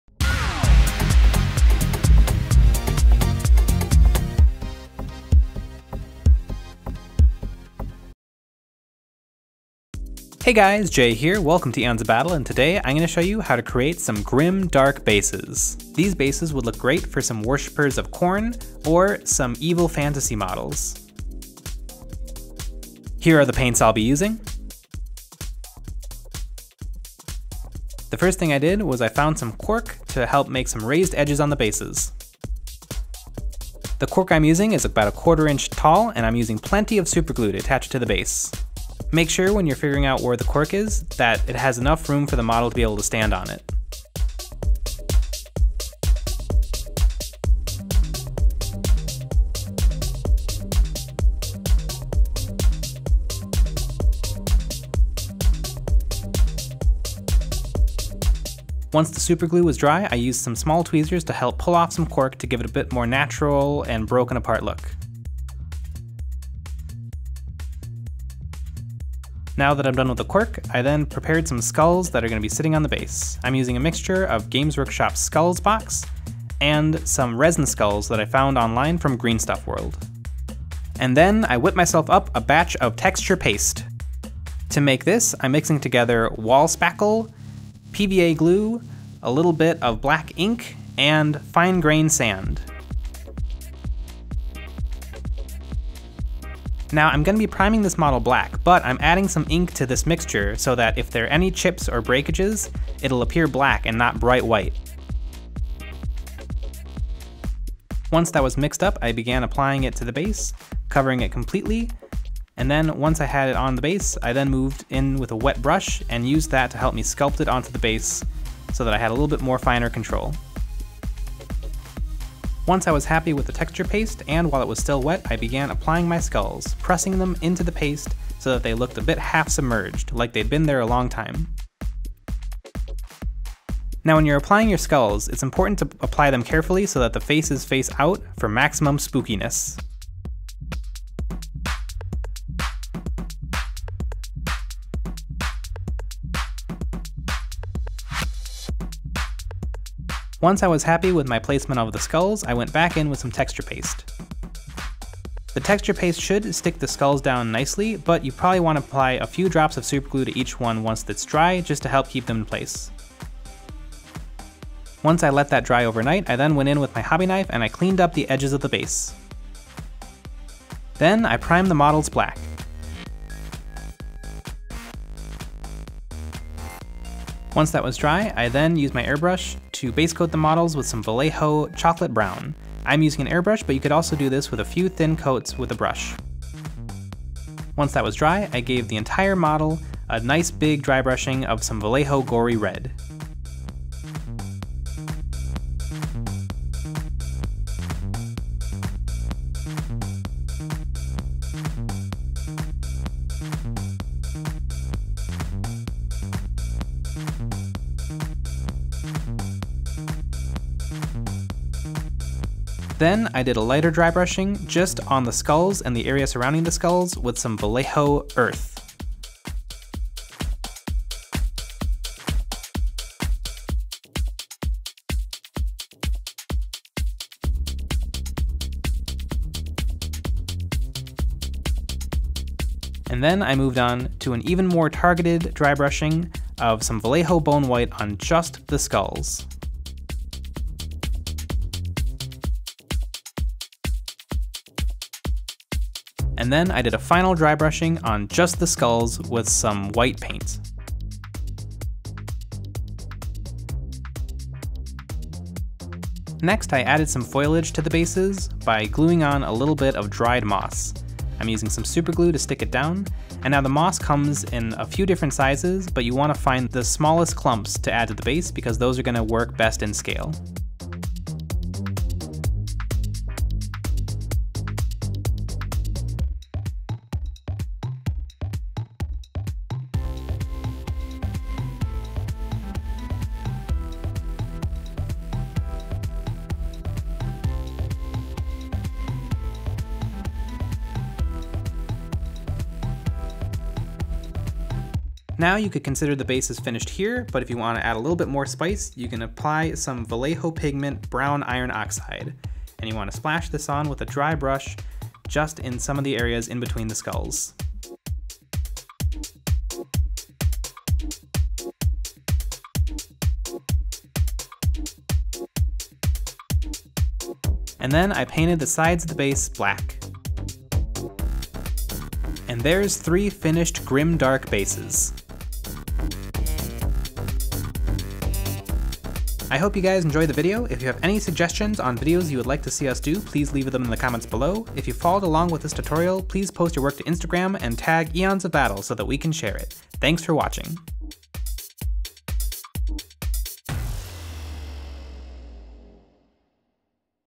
Grim Dark Base | HELLish Landscape for your Models
In this video Jay shows you how to create a Grim Dark Base! These dioramas are perfect for Khorne and other spooky fantasy models!